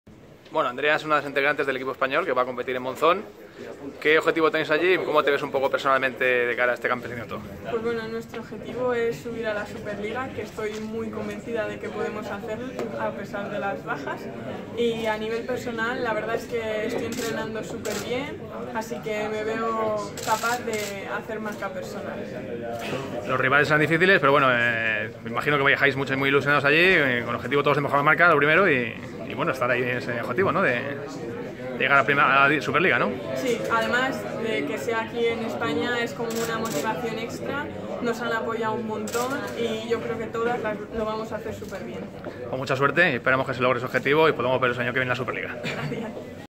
Andrea Medina ante la Copa de Europa de combinadas
La campeona de España de heptathlón Andrea Medina será una de las integrantes del equipo español de combinadas que competirá en la Copa de Europa de Monzón.